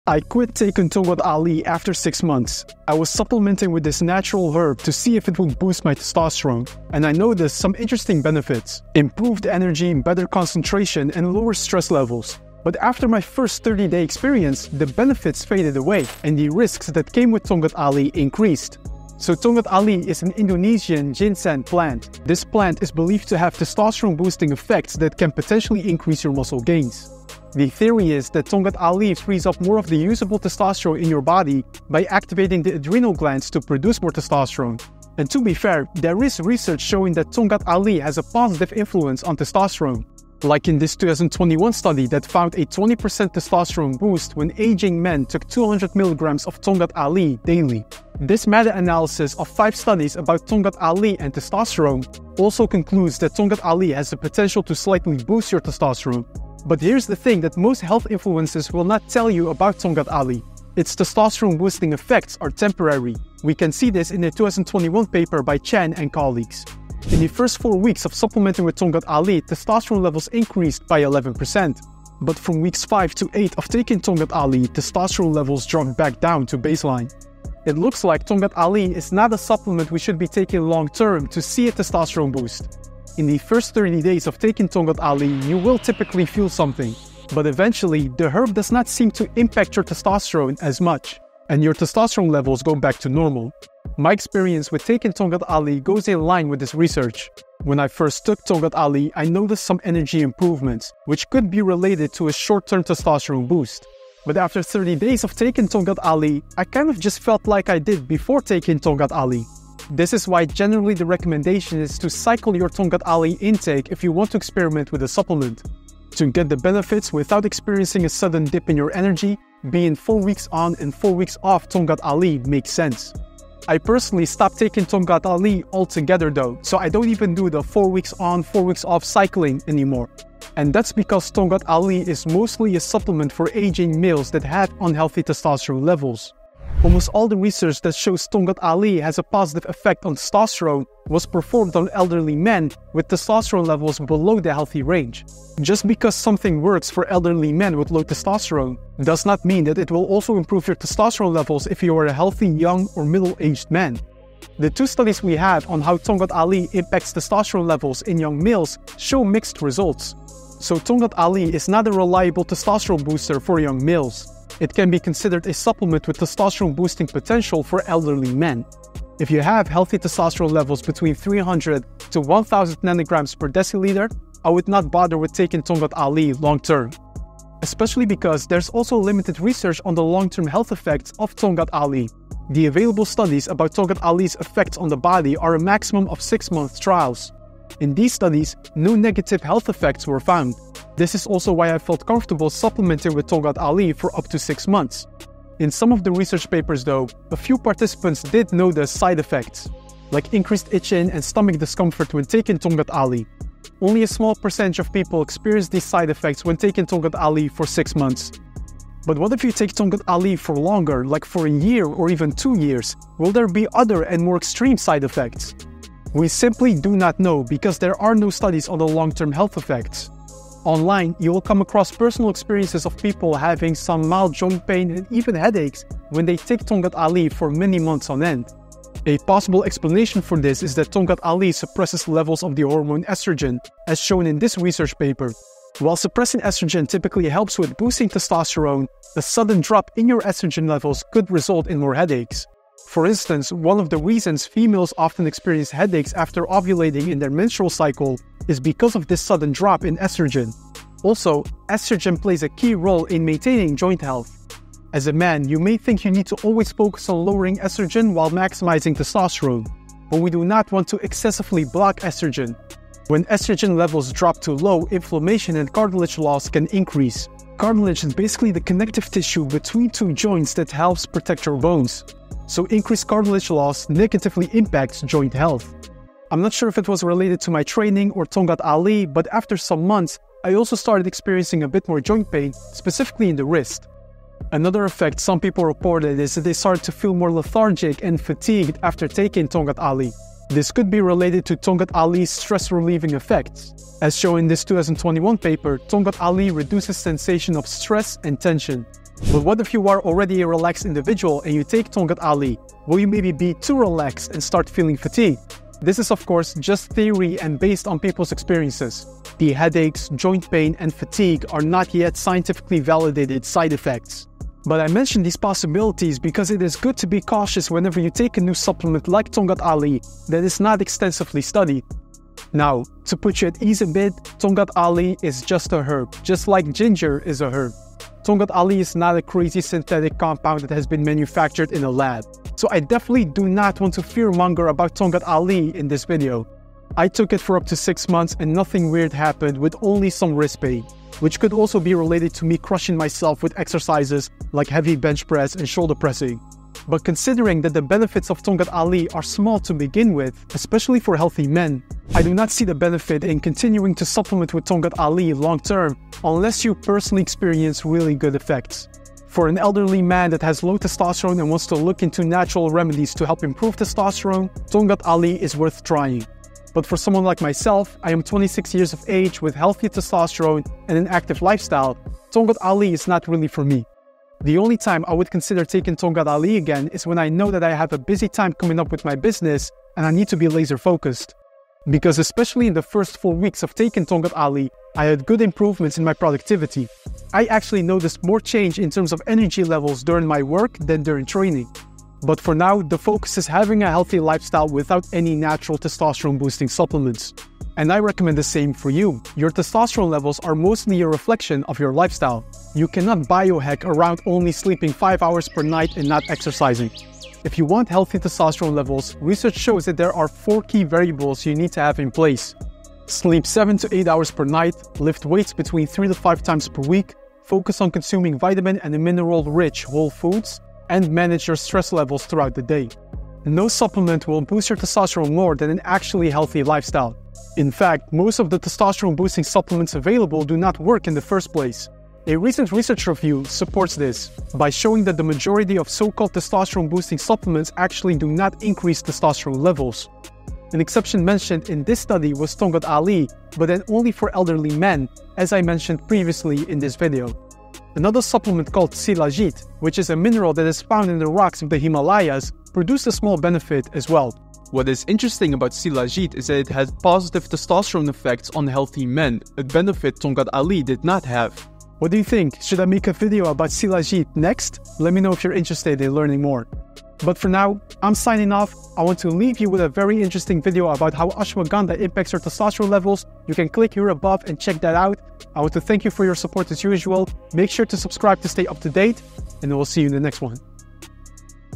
Why I STOPPED Taking Tongkat Ali (Testosterone Drops?)
I QUIT taking Tongkat Ali after 6 months. I was supplementing with this natural herb to see if it would boost my testosterone, and I noticed some interesting benefits. Like improved energy, better concentration, and lower stress levels.

But after my first 30-day experiment, the benefits faded away. And the risks that came with taking Tongkat Ali increased.

So Tongkat Ali is an Indonesian Ginseng plant. This plant is promoted as a testosterone-boosting supplement that can potentially increase your muscle growth.

Learn more about Tongkat Ali and why I don't recommend most people take this supplement long-term in thie video. See the timestamps below!

0:00-0:18 Quitting Tongkat Ali
0:19-0:56 About Tongkat Ali
0:57-2:09 The Benefits Are Temporary
2:10-2:57 Does Not Work For Healthy Men?
2:58-3:40 Health Risks?
3:41-4:10 Headaches
4:11-4:43 Joint Pain
4:44 -5:22 Fatigue
5:23-6:13 My Experience
6:14-6:38 Would I Take Tongkat Ali Again?
6:39-7:07 Testosterone Perspective
7:08-7:46 NEW Research
7:46-8:01 Conclusion